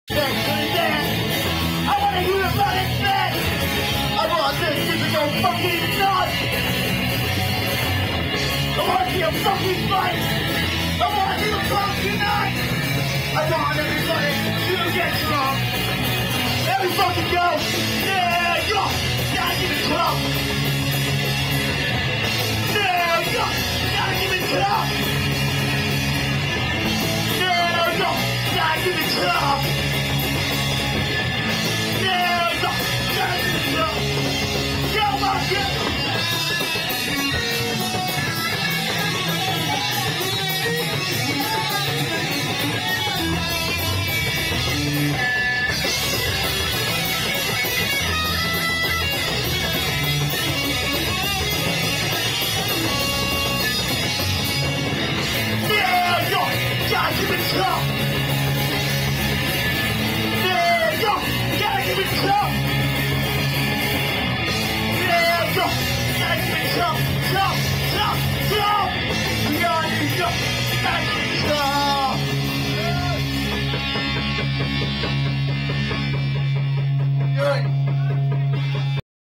Nice Punk Sample - Shirtless Male
Nice sample here in a great performance and a tight band, John Pinhead producer, Constance Pinhead, Brian Public Disturance,  Roy J camera.